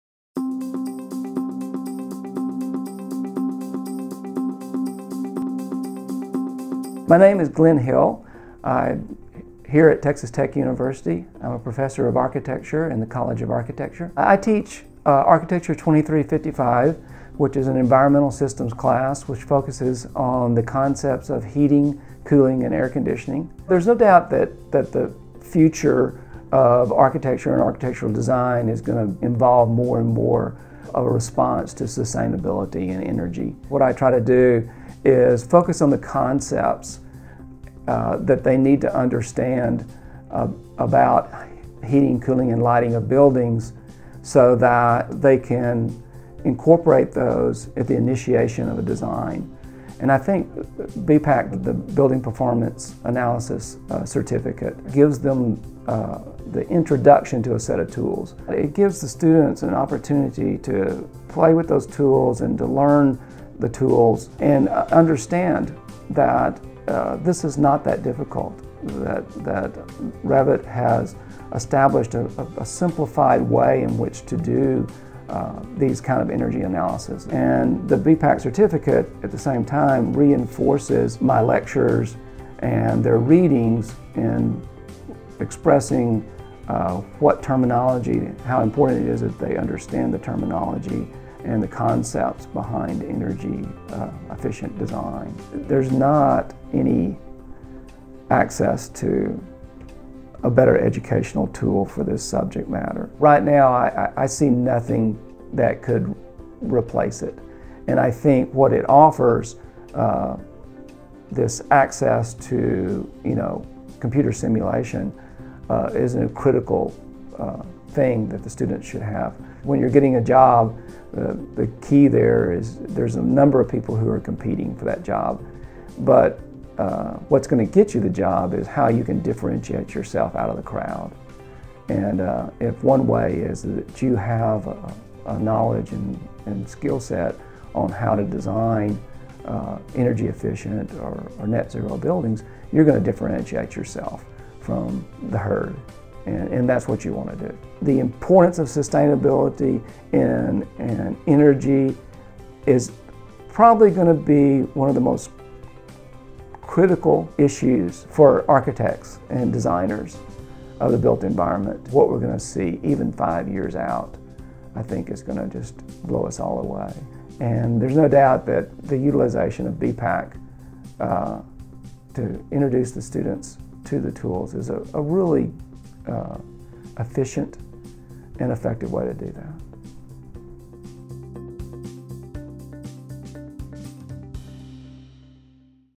Sustainable Building Design Educator Success Story, Glenn Hill
Glenn believes there is no better educational tool, than the BPA Certificate Program, for teaching his students about things like heating, cooling, and lighting of buildings so that they can incorporate those at the initiation of a building design, and that BPAC will be the differentiator for students, when they are ready to pursue jobs.